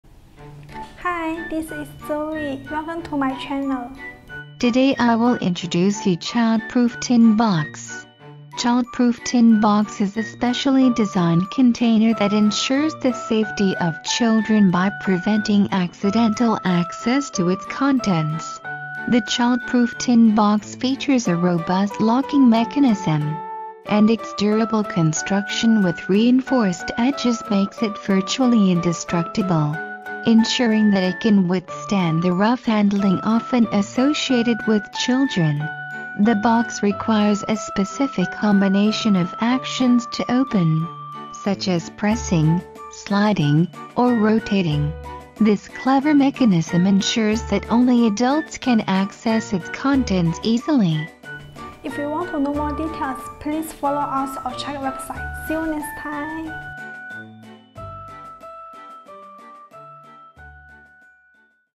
Custom child proof tin box | One-Stop Packaging Solution
child proof tin box

Today I will introduce you Child Proof Tin Box.

Child Proof Tin Box is a specially designed container that ensures the safety of children by preventing accidental access to its contents.

The Child Proof Tin Box features a robust locking mechanism, and its durable construction with reinforced edges makes it virtually indestructible, ensuring that it can withstand the rough handling often associated with children.

The box requires a specific combination of actions to open, such as pressing, sliding, or rotating. This clever mechanism ensures that only adults can access its contents easily.

If you want to know more details, please follow us or check the website.